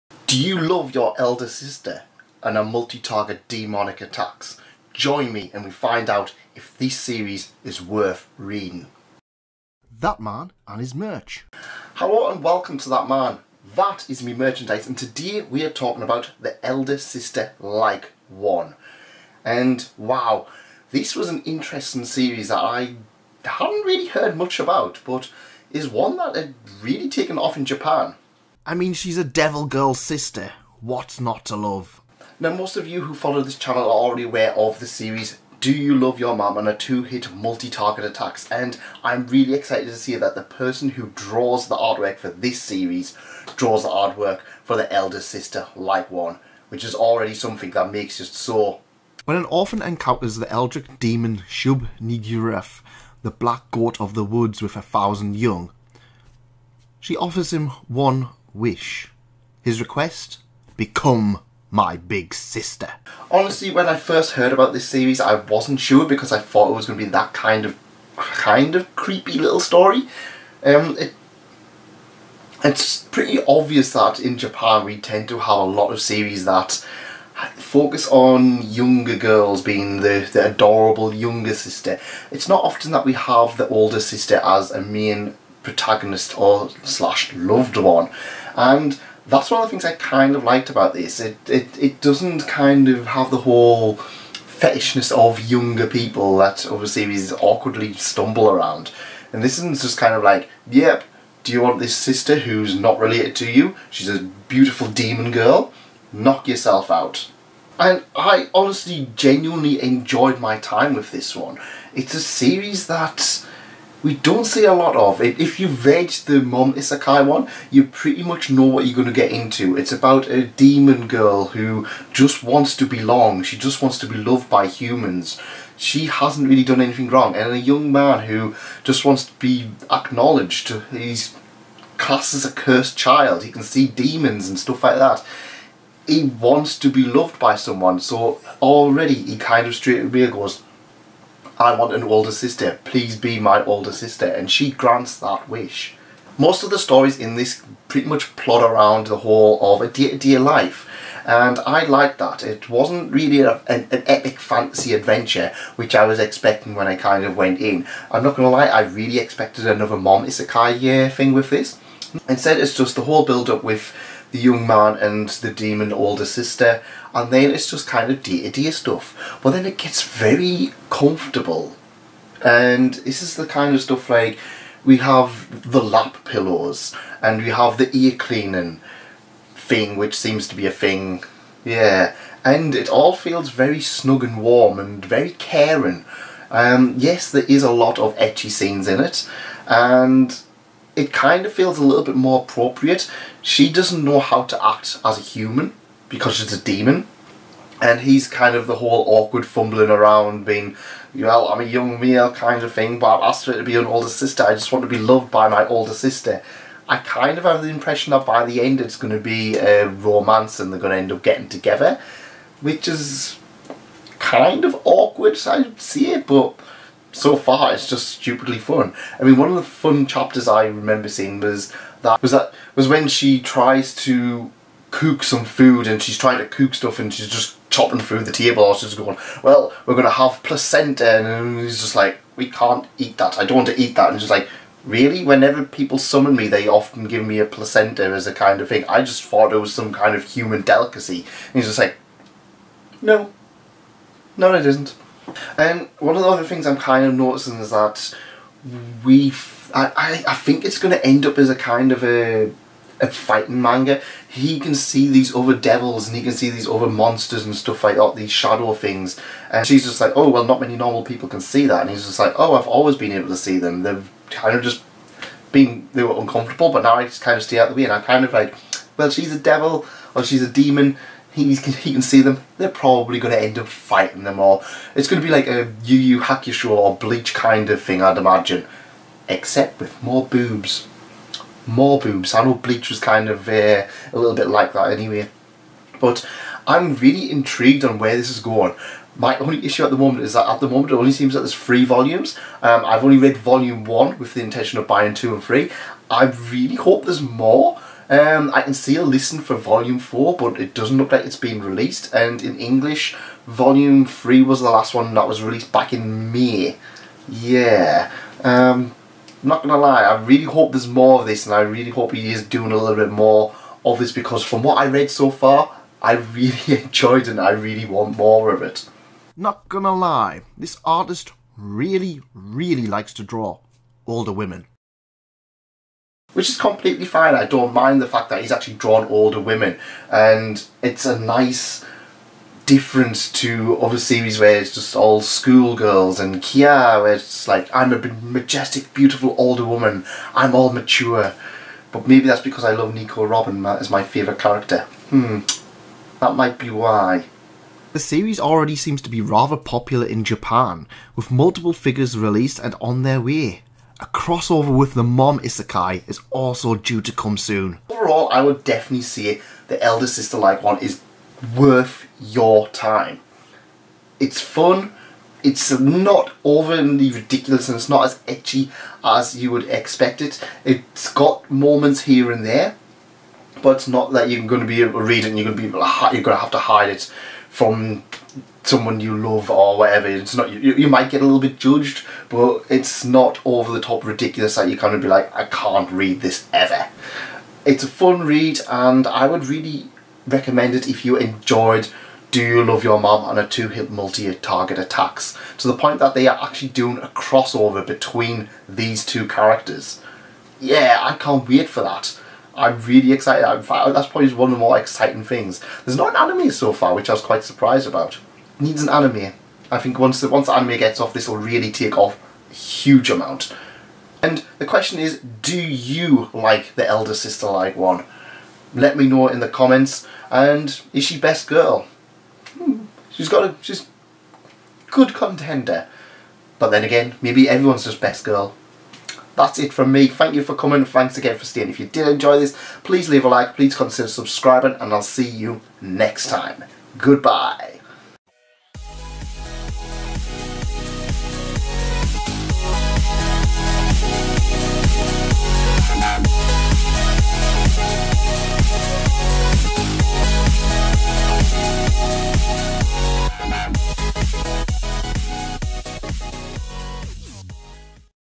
ECCHI DEMON SISTER !!? THE ELDER-SISTER LIKE ONE MANGA REVIEW
Move over Nezuko! There's a New Demon Sister In town! Join me as we look at The Elder-Sister Like One, a Manga about a young man who ends up with a beautiful older demon sister